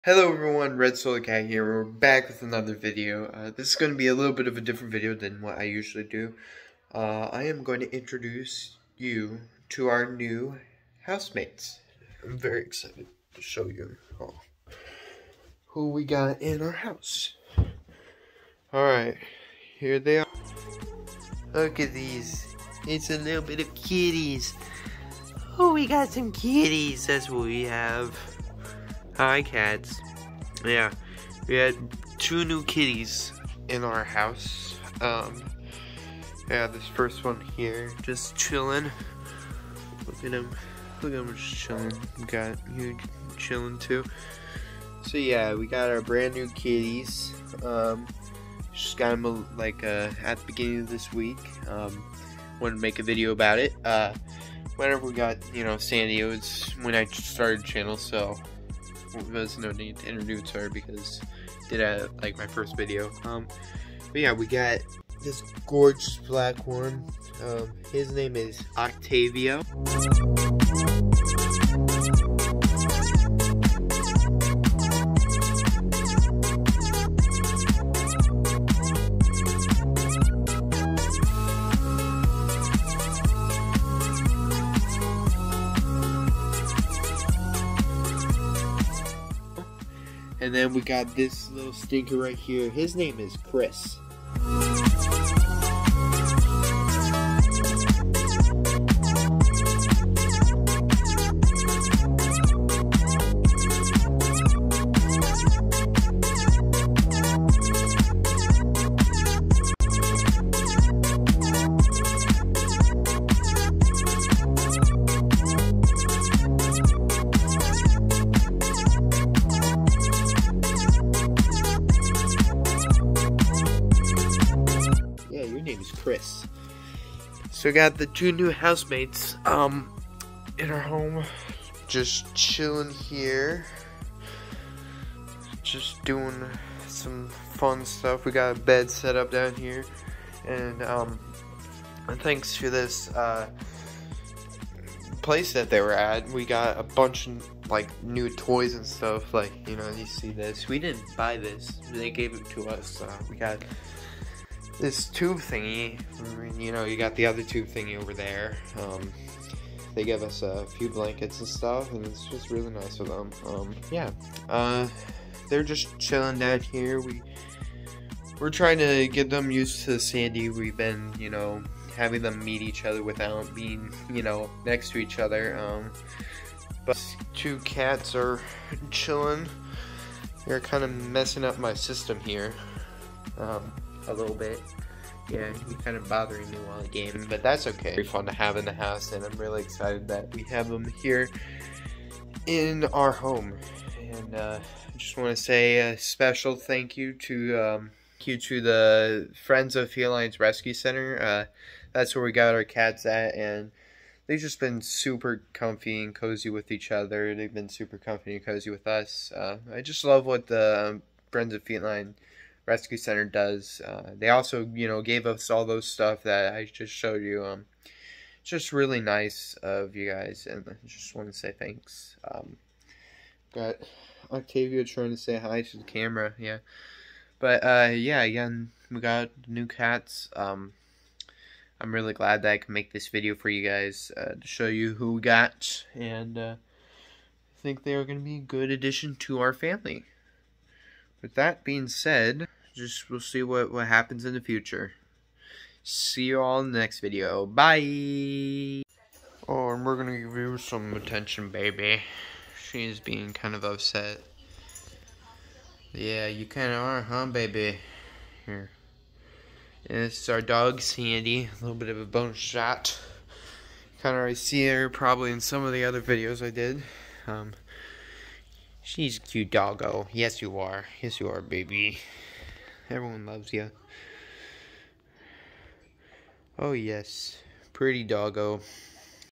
Meet the new family members!!!
Octavio and Chris were welcomed in to our family this week. We love them so much. Thanks for caring for these guys @FOFRescueCenter . Everyone there is the best!